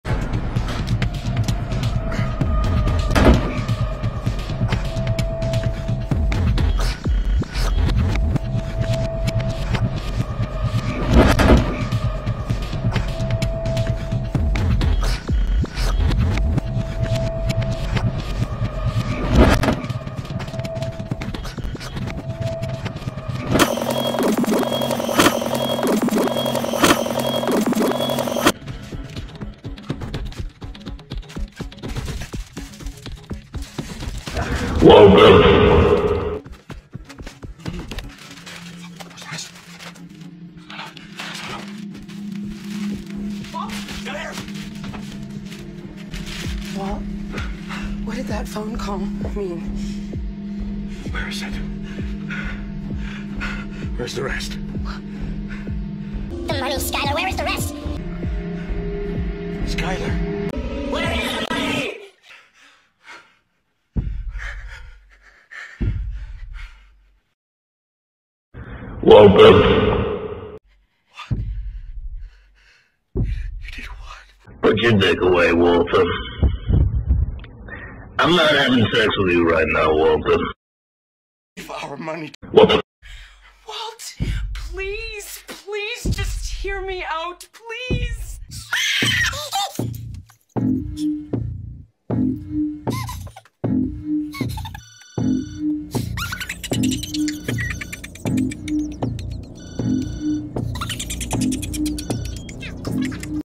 Waltuh has a stroke
Waltuh has a brain fart and now he’s asking her wife for money